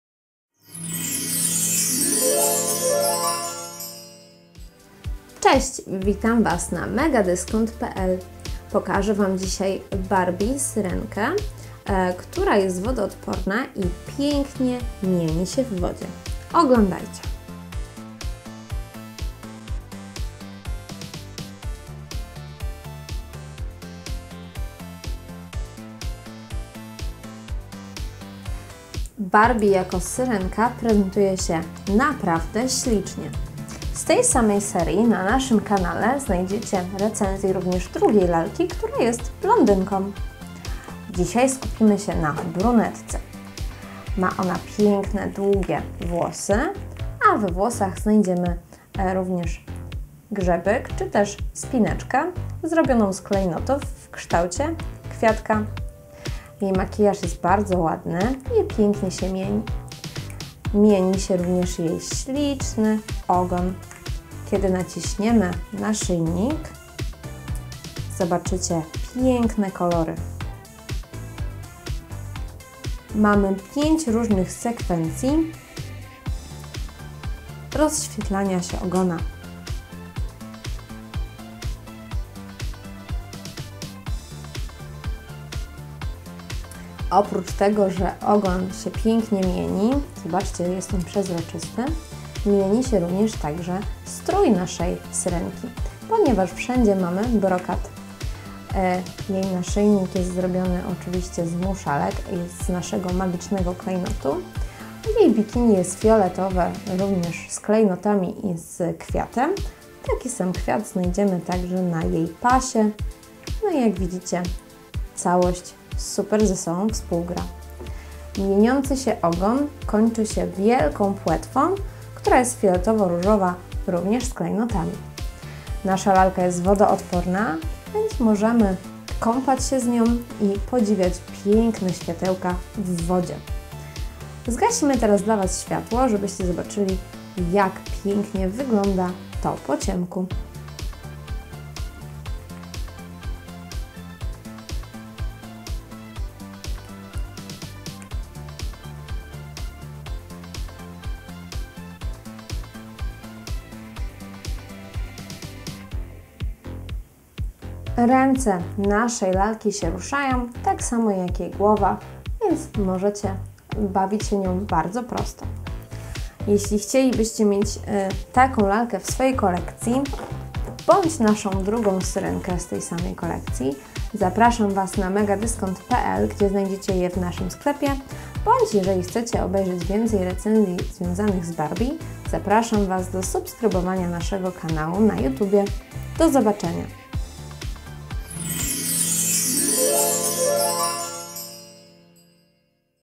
Barbie Sparkle Lights Mermaid Doll (Purple) / Barbie Fioletowa Świecąca Syrenka - V7046 CMG75
Pewnego dnia Syrenka wypływa na powierzchnię oceanu...
Ratuje młodego i przystojnego Eryka, jak się później okazuje - księcia. Syrenka bardzo chce zdobyć jego serce, za wszelką cenę chce trafić do świata ludzi. Działając pod wpływem emocji zawiera układ ze złą czarownicą morską Urszulą. Oddaje swój głos i ogon w zamian za parę nóg i szansę ponownego spotkania księcia Eryka. Działając wbrew radom swojego mądrego ojca, króla Trytona i kraba Sebastiana Syrenka popada w nie lada tarapaty ...
Dzięki nowej Syrence dziewczynki będą mogły przenieść się
w świat baśni i odtwarzać sceny z filmu!
Syrenki mają przepiękne ogony, które świecą dzięki umieszczonym w nich diodom.
Dziewczynki polubią wzory i sekwencje świateł, które będą jeszcze lepiej wyglądały w wodzie, podczas zabawy w kąpieli. Światełka uruchamia się, naciskając syrenkę w miejscu, gdzie jest naszyjnik. Lalkami można bawić się w wodzie.

Wymiary opakowania: 33 x 6 x 14 cm /szer. x głęb. x wys./.
Zabawka przeznaczona jest dla dzieci od 3 roku życia.

Barbie Sparkle Lights Mermaid Barbie Doll offers ultimate underwater fun. She's a gorgeous mermaid with a multicolored tail and floral accents. She sports long, lustrous brown hair and wears a necklace with other accessories. Kids can light up her tail with a push on her necklace.
Why You'll Love It: LIfe is better under the sea.

Age: 3 years and up

Features
Multicolored tail with floral accents
Tail that lights up
Great bath companion ⤵️

ZAPRASZAMY NA NASZ NOWY KANAŁ!